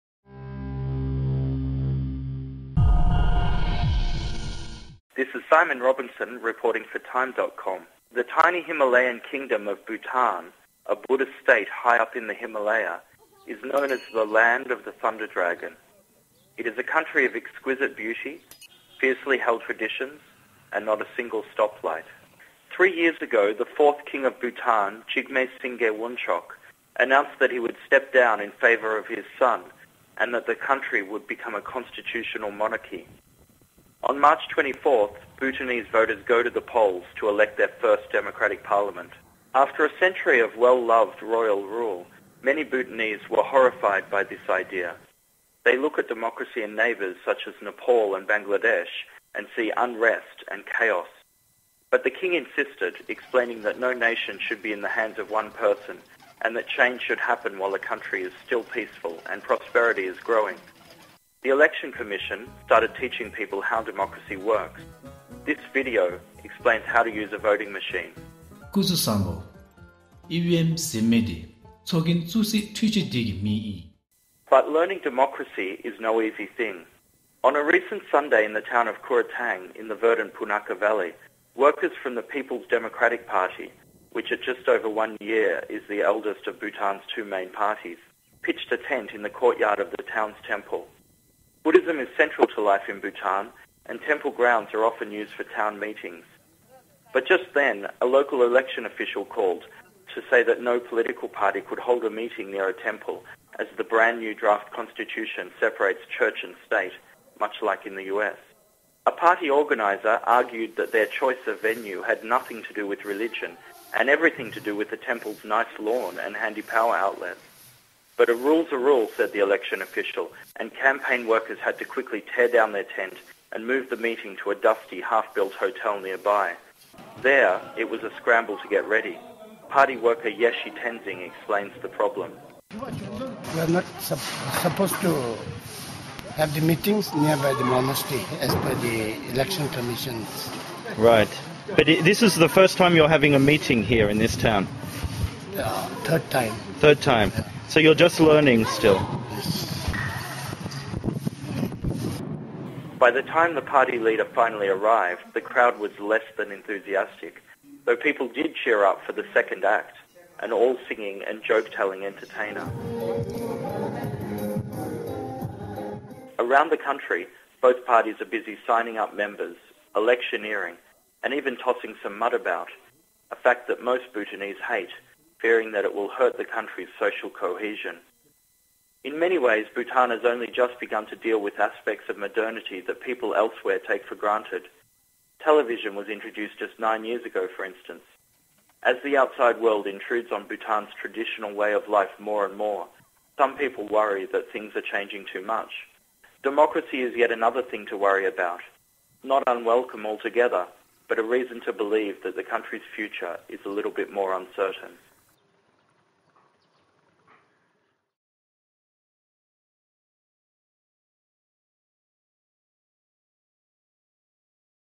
Democracy Arrives In Bhutan | TIME
After more than a century of monarchy in Bhutan, voters will soon go to the polls to elect their first democratic parliament.